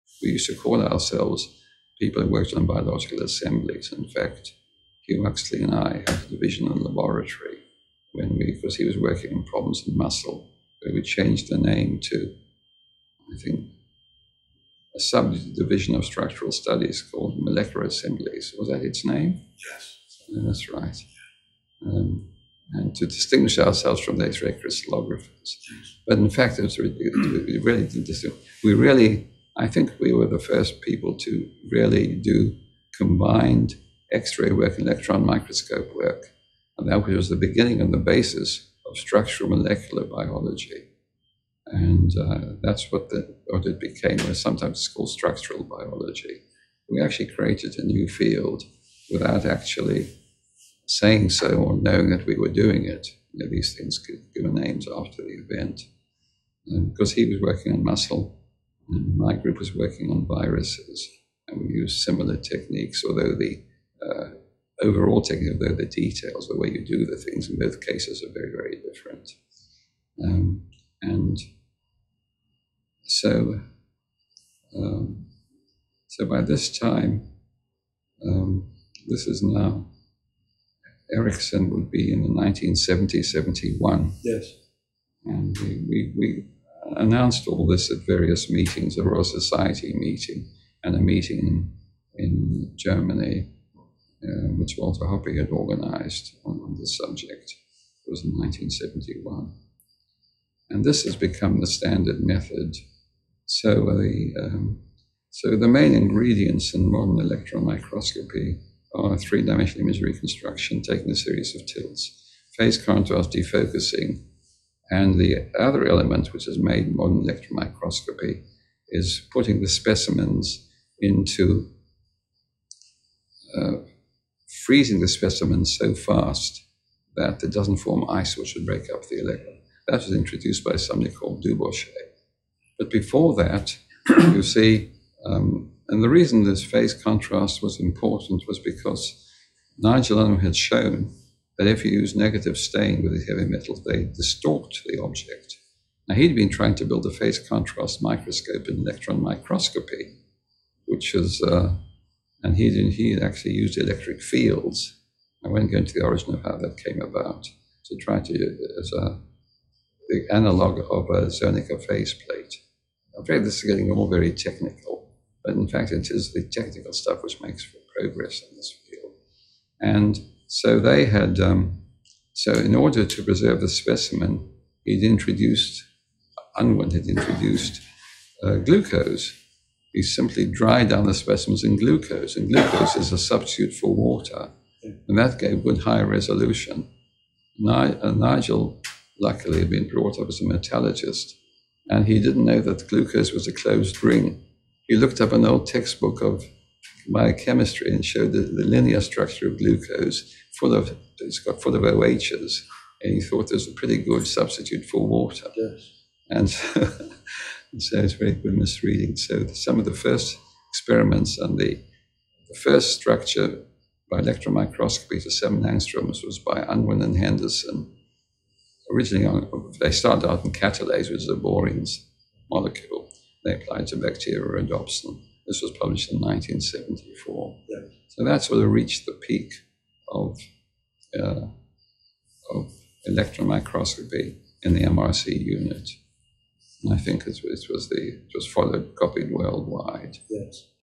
Aaron Klug - The main ingredients in modern electron microscopy (26/120)
Born in Lithuania, British chemist Aaron Klug (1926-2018) won the Nobel Prize in Chemistry in 1982 for developments in electron microscopy and his work on complexes of nucleic acids and proteins. His long and influential career led to a knighthood in 1988. [Listeners: Ken Holmes, John Finch; date recorded: 2005]

TRANSCRIPT: We used to call ourselves people that worked on biological assemblies. In fact, Hugh Huxley and I had a division in the laboratory when we... because he was working on problems with muscle, and we changed the name to a... sub-division of structural studies called molecular assemblies, was that its name?

[JF] Yes.

That's right, and to distinguish ourselves from X-ray crystallographers. Yes. But, in fact, it was very interesting, we really... I think, we were the first people to really do combined X-ray work, electron microscope work, and that was the beginning of the basis of structural molecular biology. And that's what the... what it became, it's sometimes called structural biology; we actually created a new field without actually saying so or knowing that we were doing it, you know, these things get given names after the event. Because, he was working on muscle and my group was working on viruses and we used similar techniques although the overall technique were the details the way you do the things in both cases were very, very different. And... so... so by this time... this is now... Erickson would be in the 1970, 71... Yes. And we... we announced all this at various meetings, the Royal Society Meeting and the meeting in Germany, which Walter Hoppy had organised on the subject, it was in 1971. And this has become the standard method; so... so the main ingredients in modern electron microscopy are three-dimension image reconstruction taking a series of tilts. Phase contrast defocussing and the other element which had made modern electron microscopy is putting the specimens into... freezing the specimens so fast that it doesn't form ice, which would break up the... that was introduced by somebody called [Jacques] Dubochet. But before that, you see, and the reason this phase contrast was important was because Nigel Unwin had shown that if you use negative stain with a heavy metal base they stalked the object. Now, he'd been trying to build a phase contract microscope in electron microscopy which was a... and he... then he actually used electric fields. I won't go into the origin of that, that came about to try to... the analogue of [Frits] Zernike phase plate. I'm afraid this is getting all very technical but, in fact, it is the technical stuff which makes for progress in this field. And so they had... so in order to reserve a specimen, he'd introduced, Unwin had introduced glucose. He simply dried down the specimens in glucose and glucose is a substitute for water. And that gave good high resolution... and Nigel luckily had been brought up as a metallurgist and he didn't know that glucose was a closed ring. He looked up an old textbook of my chemistry and it showed that the linear structure of glucose full of... it's got full of OHs, and he thought that was a pretty good substitute for water.

[JF] Yes.

And so... And so it's a very good mis-reading. So some of the first experiments and the first structure by electron microscopy to seven angstroms was by Unwin and Henderson. Originally, they started out in catalase, which was a boring molecule and they applied it to bacterial rhodopsin. This was published in 1974, so that's where they reached the peak of... of electron microscopy in the MRC unit. I think it was the... it was followed, copied worldwide.